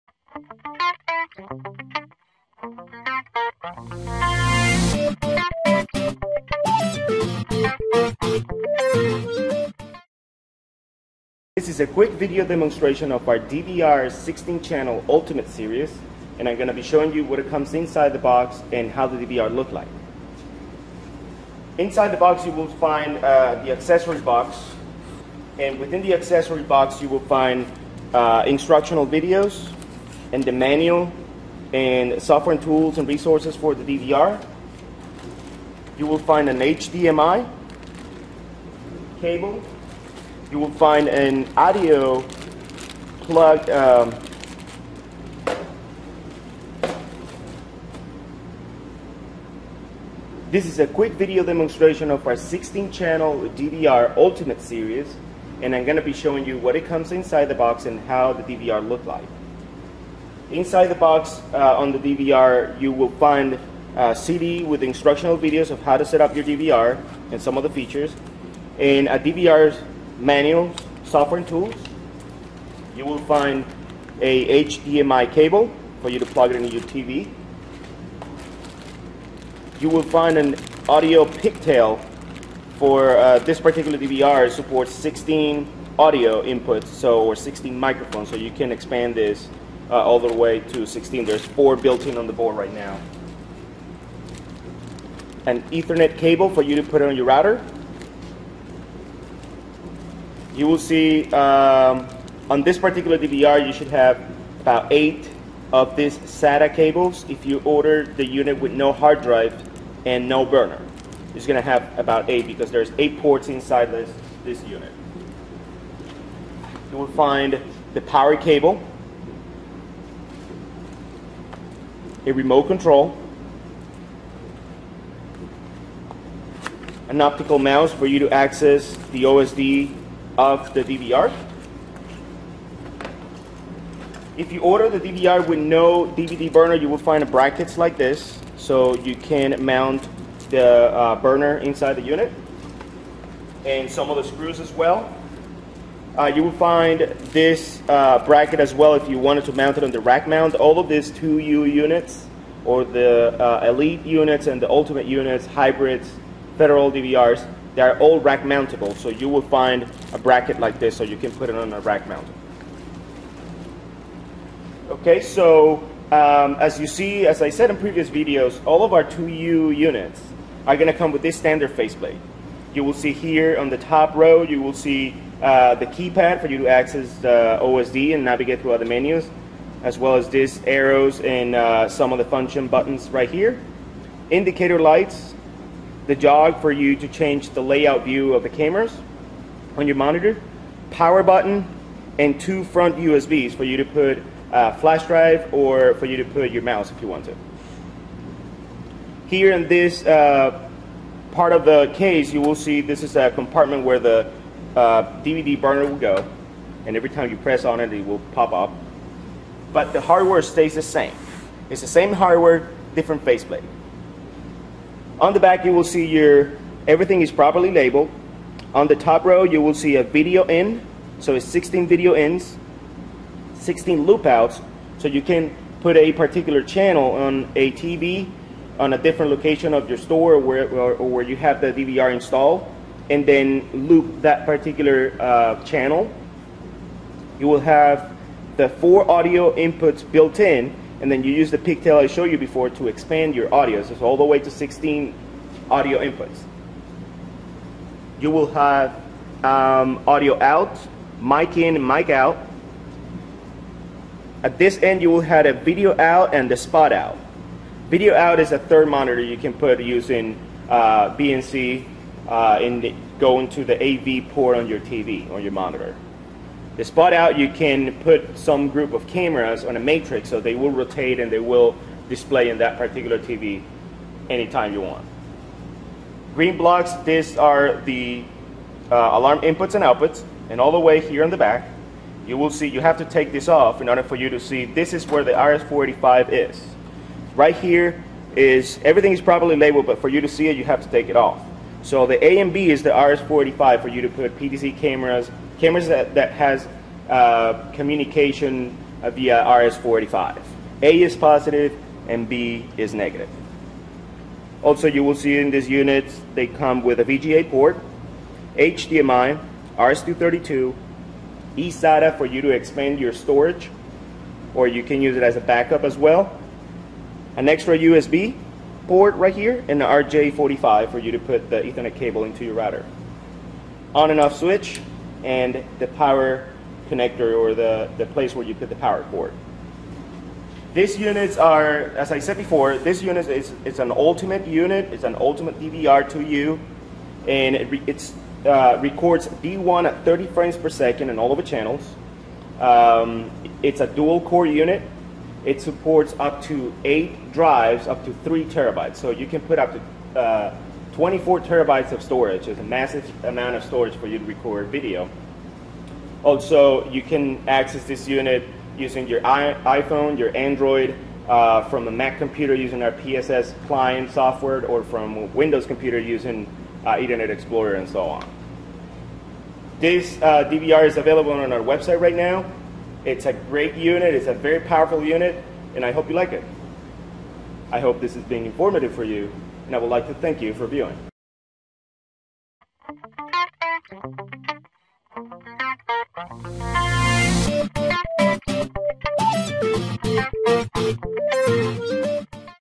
Unboxing the 16 Channel Ultimate Series H.264 D1 Realtime Security DVR - DVR-LT016480
This is a quick unboxing video of our 16CH Ultimate Series DVR. The product ID for this DVR is  DVR-LT016480.

The Ultimate DVR is just that, the ultimate in video surveillance recording. This DVR records in real-time video at 4 times the resolution of any other standalone DVR available in the industry. So if you are looking for a DVR that can record D1 quality video in real time on all channels, this is the unit you want. The Ultimate Series DVR is the best quality DVR in the industry.